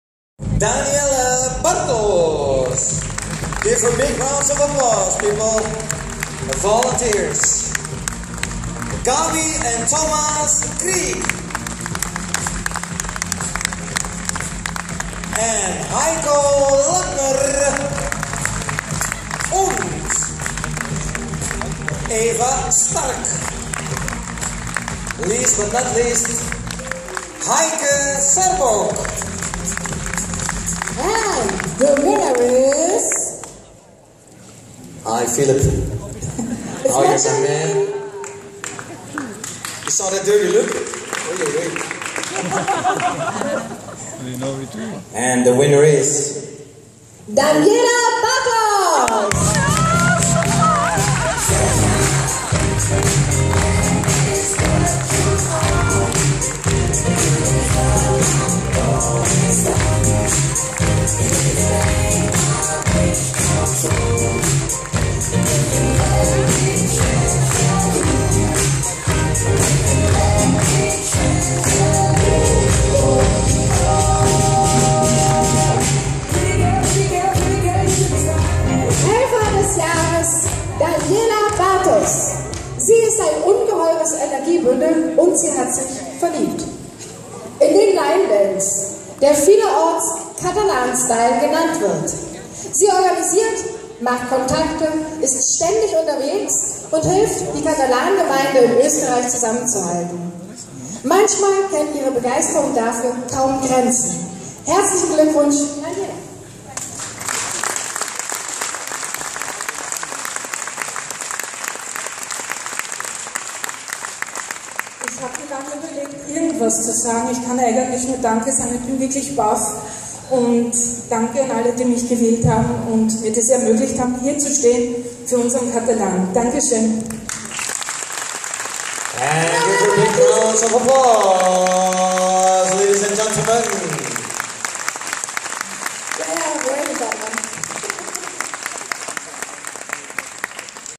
Living Linedance AWARD Auszeichnung 19.7.2014 in Kalkar
1. Austrian Fanclub for Linedance in Catalan Style
Nominiert wurde ich Daniela Bartos in 3 Kategorien
-) Helfer des Jahres
-) Event des Jahres
-) Gruppenvideo des Jahres
gewonnen habe ich dann als Helfer des Jahres, was für mich immer unvergessen bleiben wird.